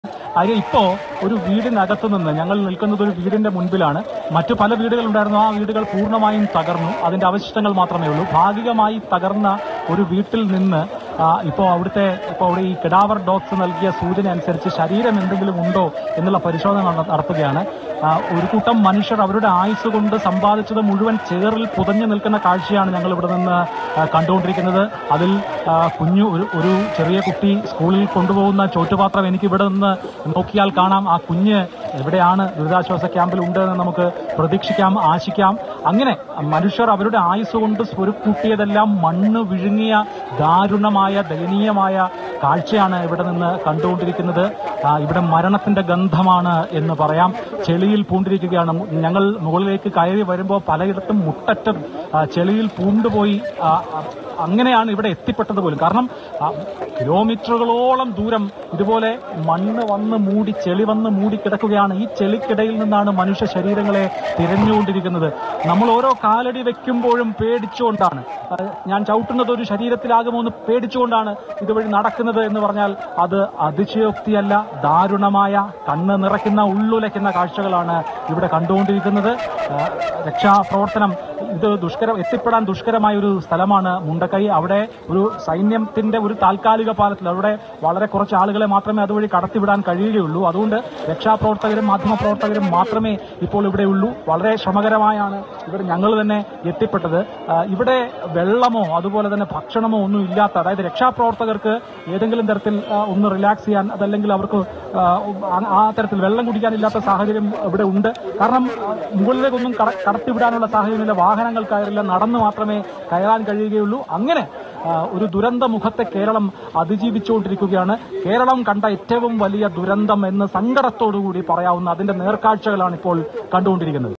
'ചവിട്ടുന്നത് ഒരു ശരീരത്തിലേക്കാകുമോ എന്ന ഭയം'; ചെളിയിൽ ജീവന്റെ തുടിപ്പ് തിരഞ്ഞ് രക്ഷാപ്രവർത്തകർ
Happy viewing!